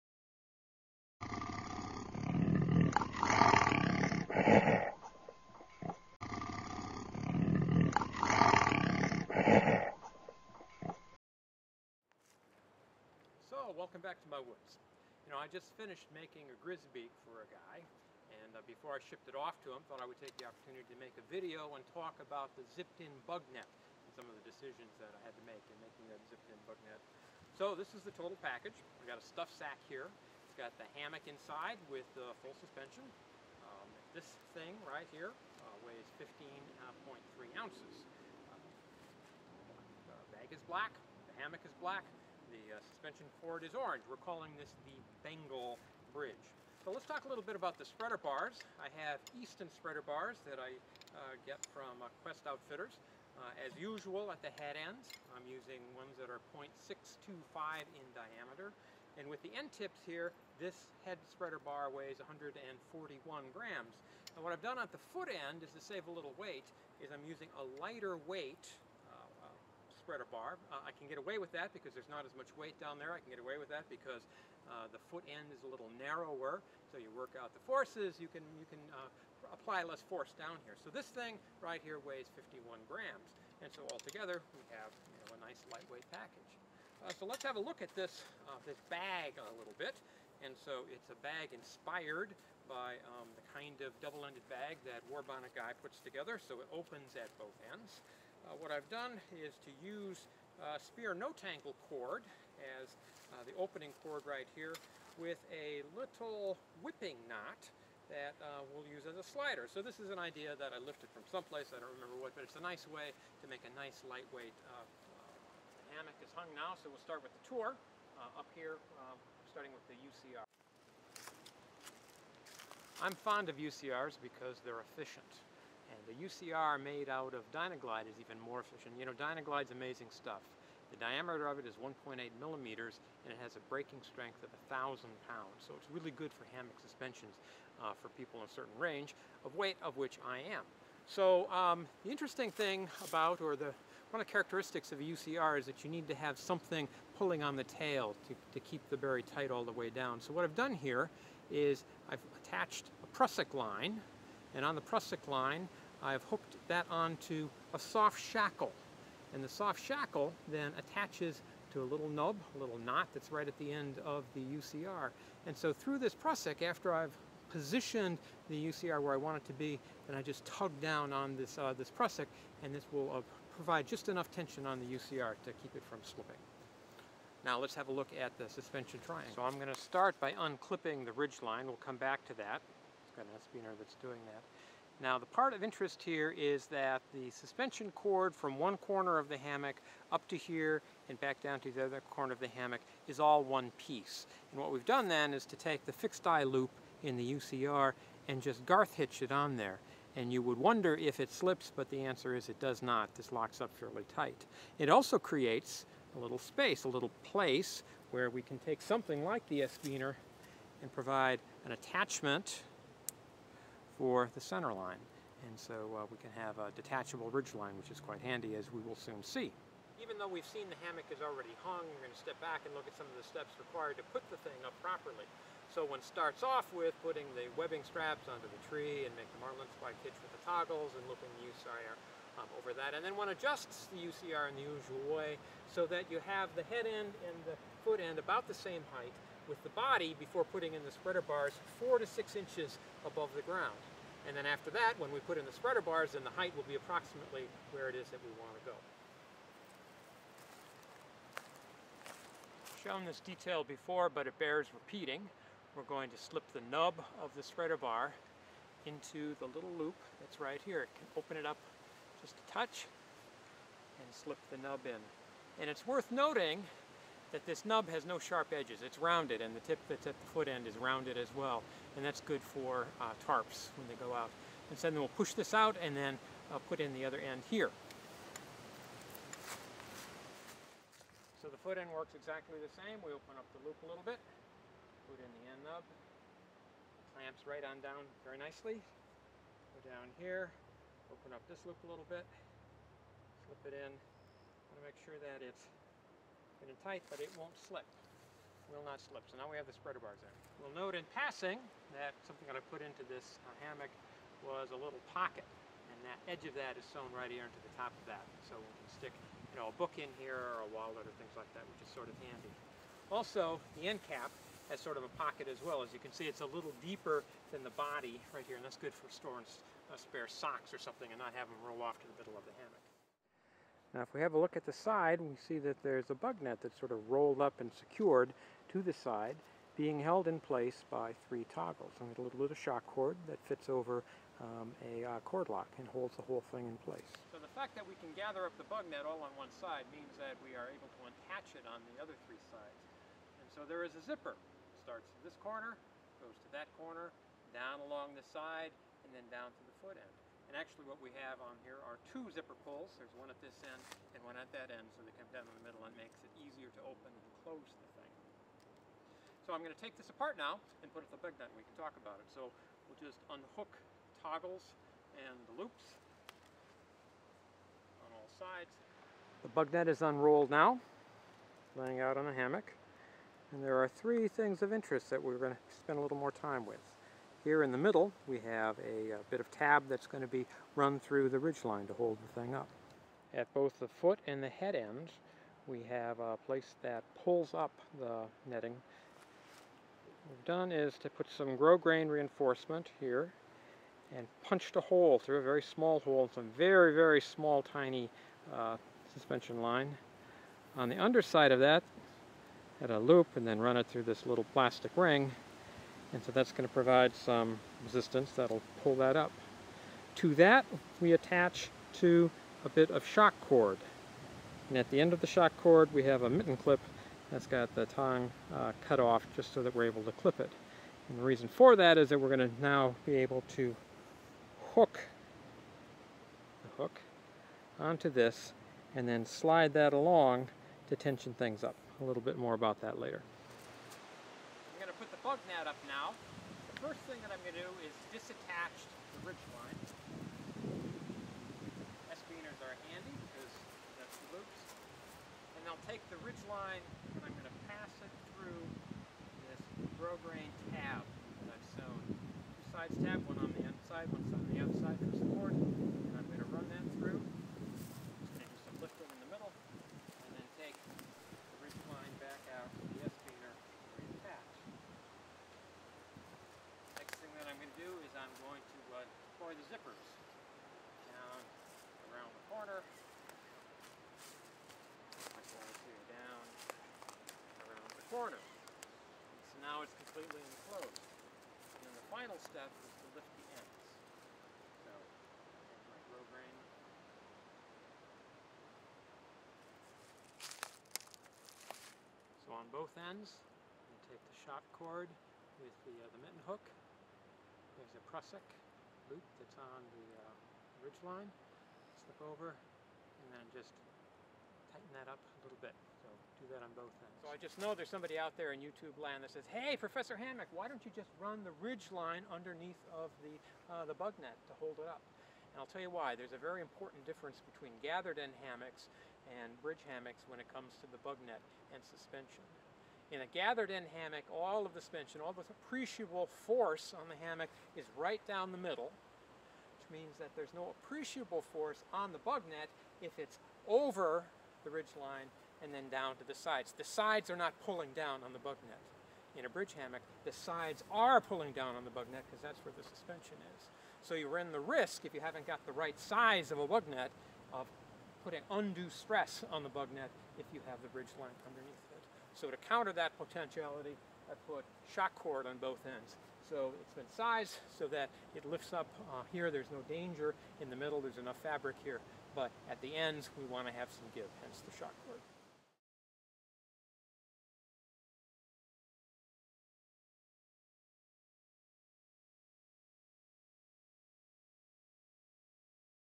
GrizzBridge Revealed! The Bengal Bridge (part 1 : set-up )
This video and its companion (part 2) look at a GrizzBridge made entirely of black 1.9 oz micro-fiber polyester, black no-seeum bug netting, and orange cord, hence the name "Bengal Bridge".  To the extent there is anything interesting in either video not already covered by a previous GrizzBridge video, it is attention to issues related to putting in a full zippered bug-net. Part 1 goes through the hammock set-up.